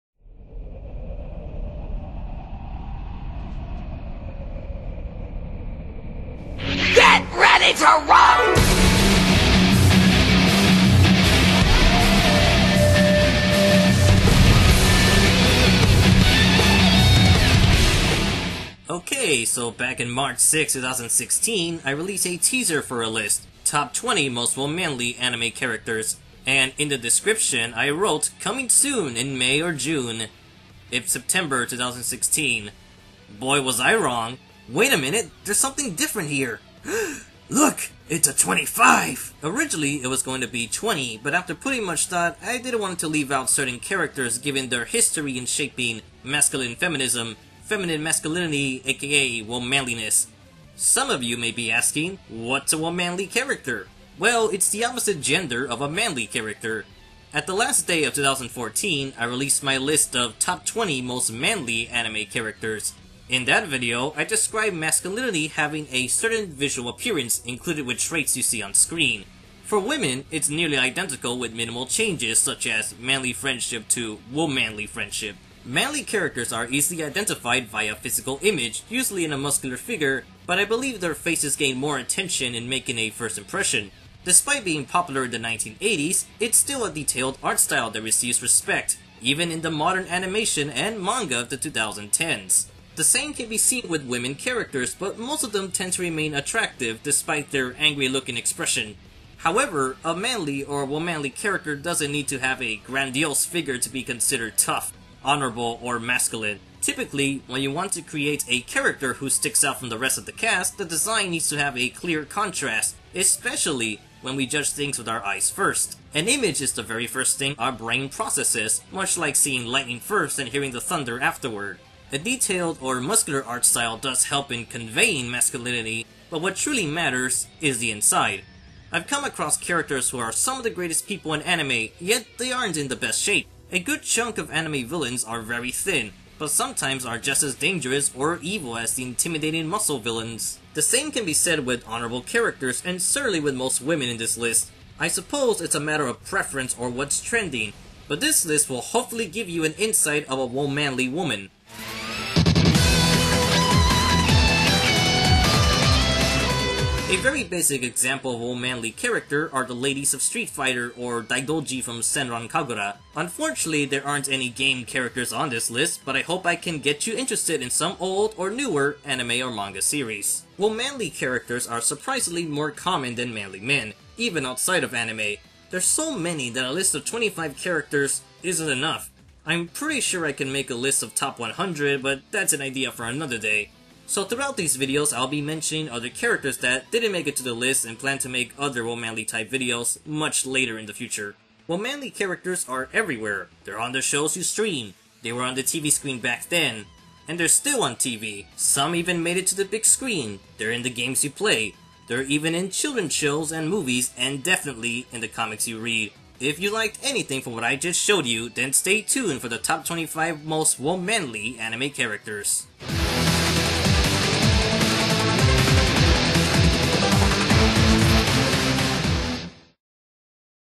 AH PREVIEW TOP 25 WOMANLY Anime Characters Prologue
This is a sample of the upcoming list of WOMANLY characters & a sequel to 2015's Top 20 Most Manly Anime Characters. I hope this leads to a increased interest in strong willed women found in anime & manga. Characters will be from any time period showing the best of the old and the new. One day a list of 100 Womanly Characters will be made but for now this is the basic guide into the unnamed genre or demographic of Womanly.